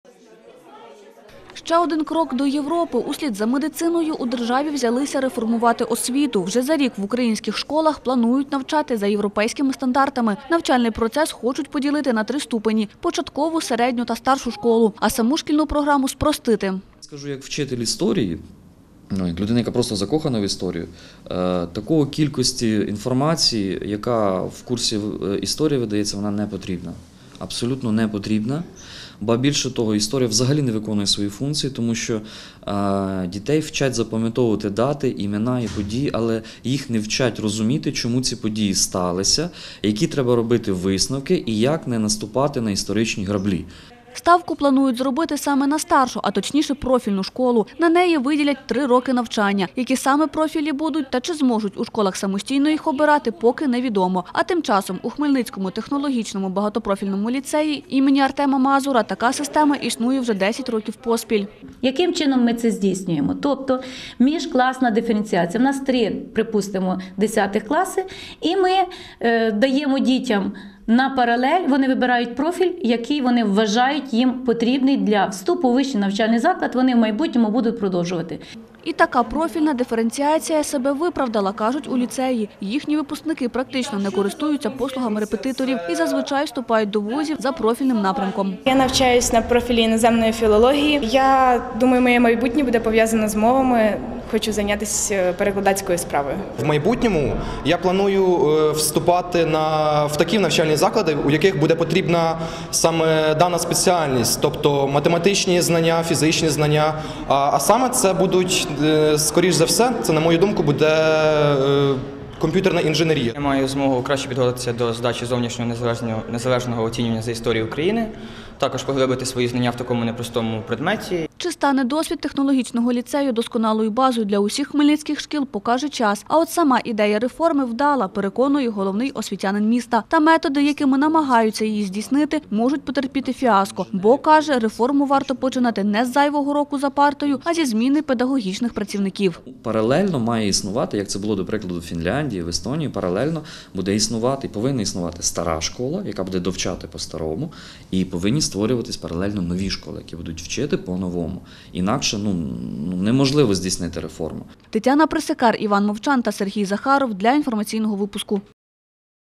Знання за європейськими стандартами чи зайвий рік у школі
Шкільна освіта довжиною 12 років. Таку реформу підготувало профільне міністерство.  Планують, що  з 2018 року майбутні першачки вчитимуться на 1 рік довше. А от  старші класи стануть профільними. Окрім того, деякі предмети об’єднають у нові дисципліни. Інші – просто зникнуть із шкільного розкладу. Чи готові до таких інновацій у школах краю, - з’ясовувала Тетяна Присикар.